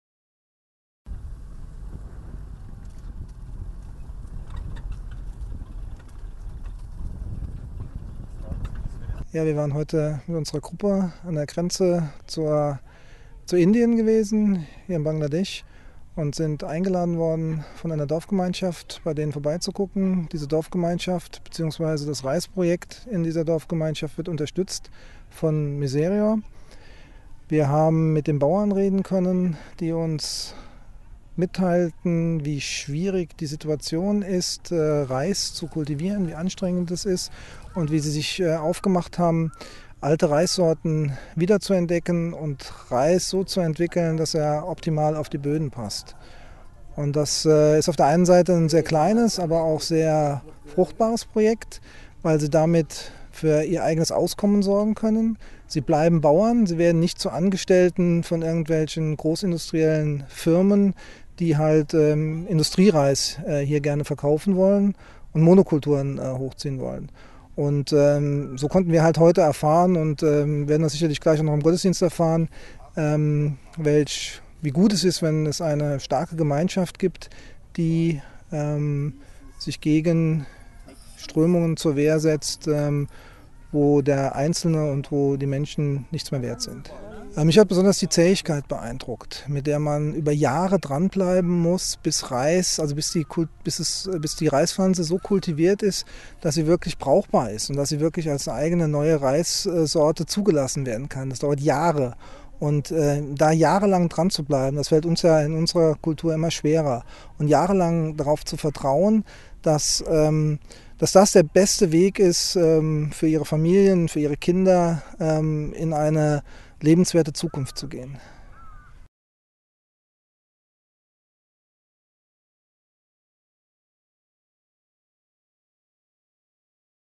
Überall Reis - Hilfe für die Reisbauern in Bangladesch
"Wir haben den Hunger satt!" Ist das Leitwort der MISEREOR-Fastenaktion 2013, Es geht unter anderem darum, dass Hunger kein Schicksal ist, sondern ein überwiegend von Menschen gemachtes Problem. Dass es auch anders möglich ist, zeigt ein von MISEREOR unterstütztes Projekt von Caritas Bangladesch - ganz oben im Norden des Landes, an der Grenze zu Indien. Dort haben Kleinbauern in ihren Dörfern wieder angefangen, ihre alten traditionellen Reissorten neu zu kultivieren und daraus neue Sorten zu züchten. Und das sogar umweltschonend ohne Dünger und Pestizide. Die Bauern werden unabhängig von den großen Konzernen und weniger empfindlich, was die Folgen des Klimawandels angeht. Außerdem entwickeln sie ein ganz neues Selbstbewusstsein, denn sie schaffen es aus eigener Kraft, dem Hunger zu trotzen und sind keine Almosenempfänger.  Unser Besuch hat uns das eindrucksvoll gezeigt.